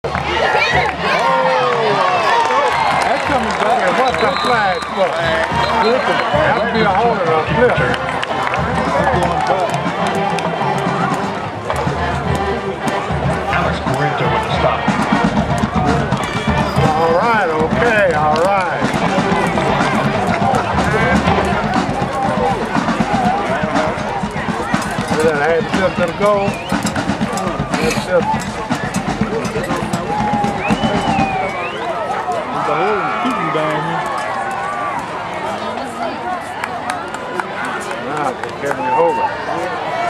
NHS Band Jaming
Nacogdoches high school band jams in the stands 10-11